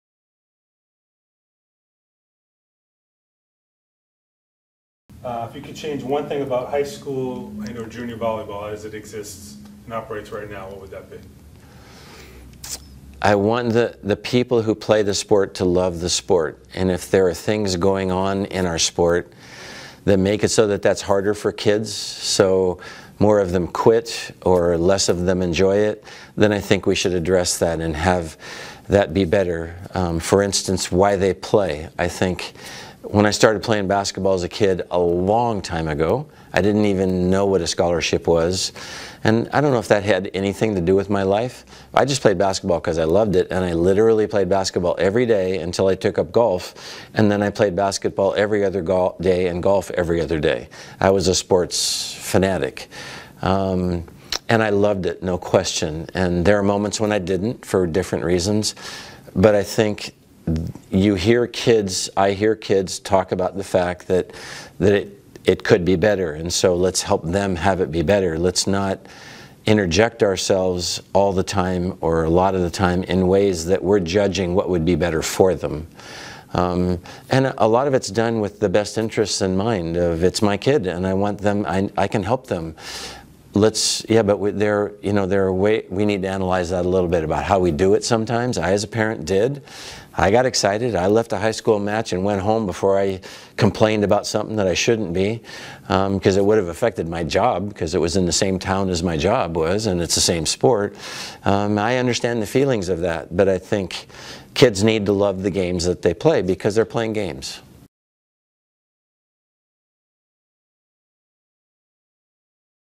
John Dunning on Improving youth volleyball
John Dunning is the recently retired Womens' Volleyball Head Coach at Stanford University. He,took over the program in 2001. During his tenure his teams won over 400 matches and three NCAA Division I titles. This follows 16 years at Pacific where he also won more than 400 matches and collected a pair of National Championships. Every one of John's teams reached the NCAA tournament and all together they have reached the the title match 10 times. He's won numerous Coach of the Year awards and is an inductee into the American Volleyball Coaches Association (AVCA) Hall of Fame.

In this clip from a joint interview effort between the Art of Coaching Volleyball and the Volleyball Coaching Wizards project, John discusses how youth volleyball - youth sports in general - can be improved.

The interview was conducted by John Forman, who authors the blog Coaching Volleyball

Information on the Art of Coaching Volleyball can be found